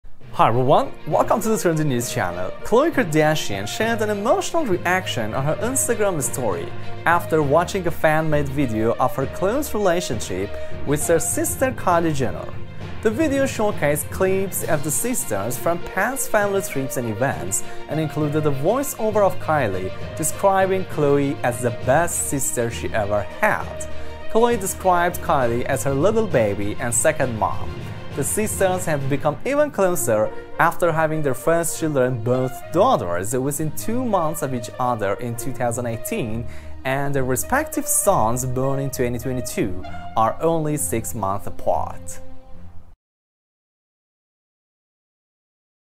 Khloe Shares Emotional Reaction to a Video Montage of her Relationship with Sister Kylie Jenner
Khloe Kardashian
shared an emotional reaction
on her Instagram story
after watching
a fan-made video
of her close relationship
with her sister Kylie Jenner.
The video showcased
clips of the sisters f
rom past family trips and events,
and included a voiceover
of Kylie describing Khloe
as the best sister she ever had.
Khloe described Kylie
as her "little baby"
and "second mom."
The sisters have become even closer
after having their first children,
both daughters,
within two months of each other in 2018,
and their respective sons,
born in 2022, are only six months apart.